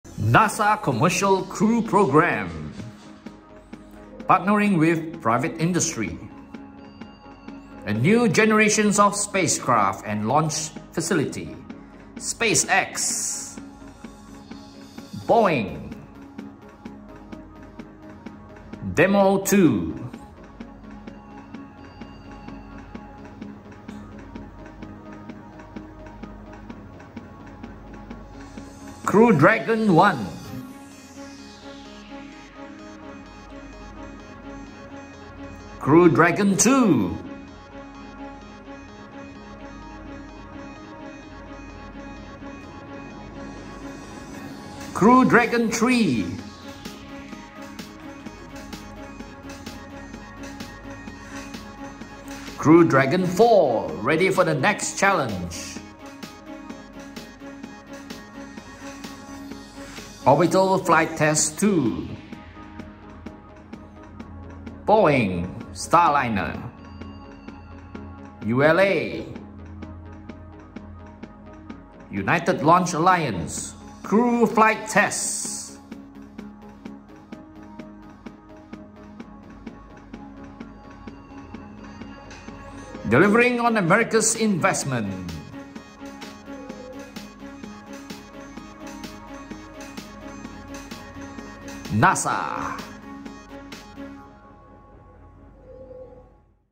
NASA 2022 Commercial Crew Program - Embracing the Next Challenge
NASA's 2022 Commercial Crew Program (CCP), based at the agency's Kennedy Space Center in Florida, eclipsed several milestones in 2021. This year 2022, CCP will continue to partner with private industry in embracing the next challenge.